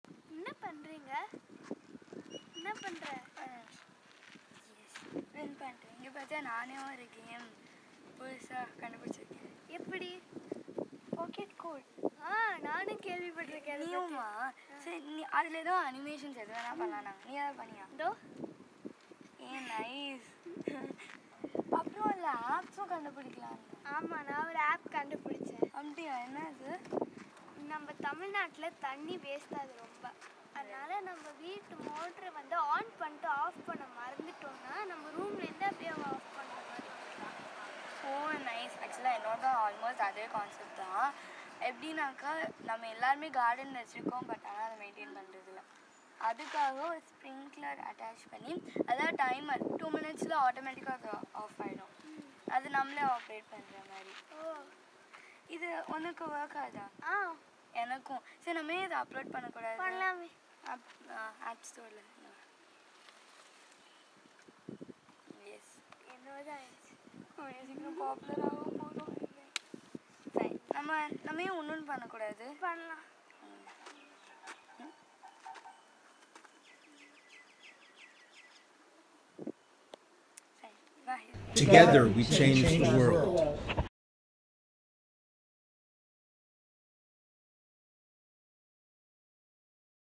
Disruptive innovation can lift nations out of poverty! (our 100&Change submission video)
We help eradicate poverty, starting in India. A sustainable prosperity depends on an economy of innovation, which in turn depends on creativity and technical skills.  Smartphones with Internet enable even young people to sell apps to a worldwide market. Our already collaborating team includes the Play Learn Foundation India, with a decade of experience teaching especially rural youth and girls literacy and app programming skills, and three university partners (MIT, Berkeley, and Graz University of Technology) who are the developers of three complementary visual programming platforms (App Inventor, Snap!, and Catrobat) that enable intuitive first steps to allow anyone to develop code using their native language.  All of the young people we work with develop computational thinking skills and an innovation mindset, which will benefit them in their careers and entrepreneurial endeavors; some will gain immediate reward through worldwide app sales. In post-grant years we’ll extend this work beyond India.

Girls chatting in the Video:
Sangamithra (Red Dress seated on the left)
Aiswaraya (White dress seated on the right)

Representatives of the partners in the video:
Aditi Prasad, Play Learn Foundation India - Indian Girls Code
Harold (Hal) Abelson, Massachusetts Institute of Technology - MIT App Inventor
Brian Harvey, University of California, Berkeley - Snap!
Bernadette Spieler, Graz University of Technology - Catrobat/Pocket Code